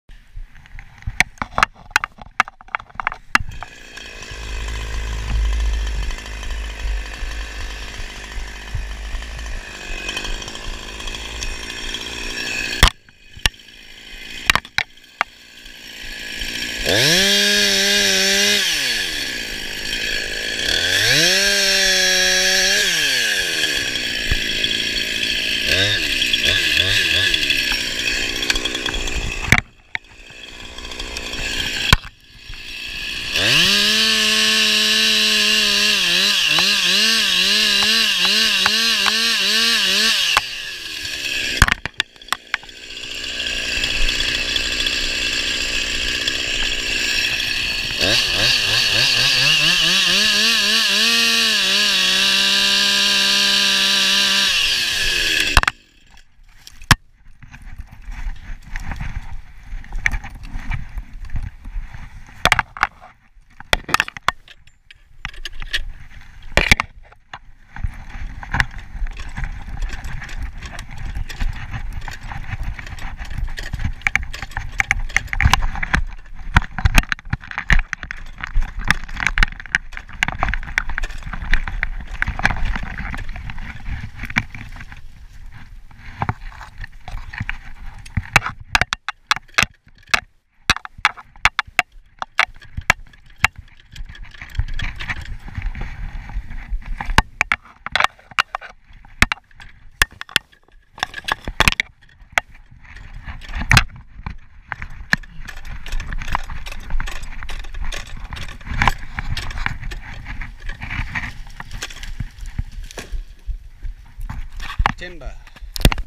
Husqvarna 346xp Trio Brake one man winch felling sitka spruce
Husqvarna 346xp Trio Brake one man winch felling sitka spruce, these spruce were being removed as part of a thinning project so the tree canopy was quite packed meaning these two spruces would have hung up, so instead of getting them hung up to then winch out I used a one man winch system to safely fell the trees.